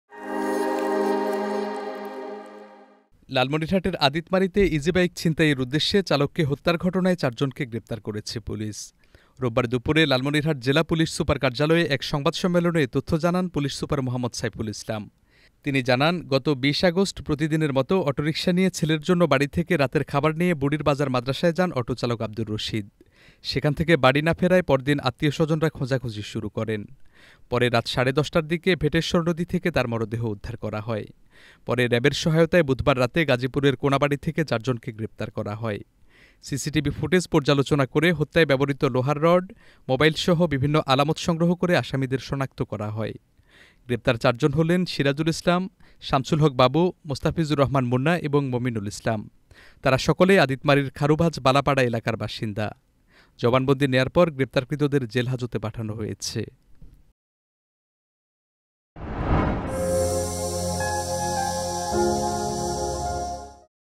লালমনিরহাটে ইজিবাইক চালককে হত্যার ঘটনায় চারজন গ্রেপ্তার | Channel S News | Channel S 2023
লালমনিরহাটের আদিতমারীতে ইজিবাইক ছিনতাইয়ের উদ্দেশ্যে চালককে হত্যার ঘটনায় চারজনকে গ্রেপ্তার করেছে পুলিশ। রোববার দুপুরে লালমনিরহাট জেলা পুলিশ সুপার কার্যালয়ে এক সংবাদ.

1: News :
2: Entertainment News :
3: Sports News :